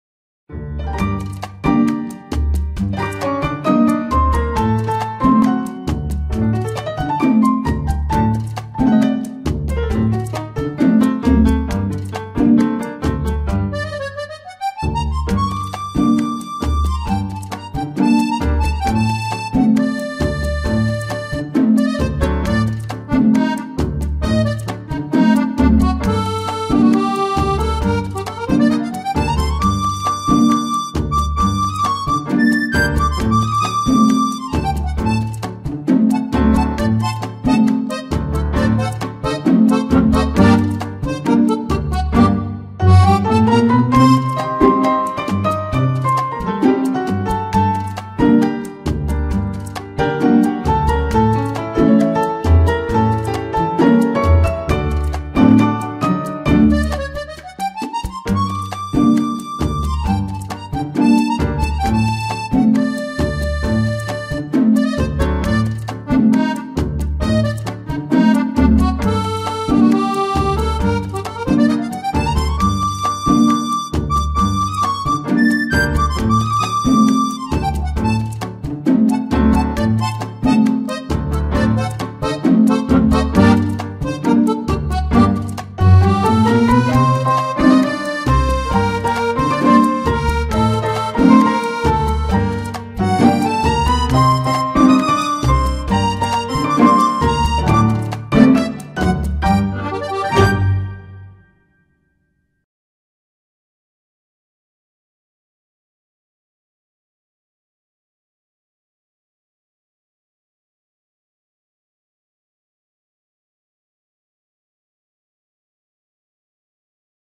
Tango
Zdjęcia można łatwo przetworzyć w taki sposób, aby przypominały malowane ręcznie obrazy. Potem podłożyć muzykę i jest gotowy filmik.
Jak to zrobić?
Zapraszam do obejrzenia poradników na kanale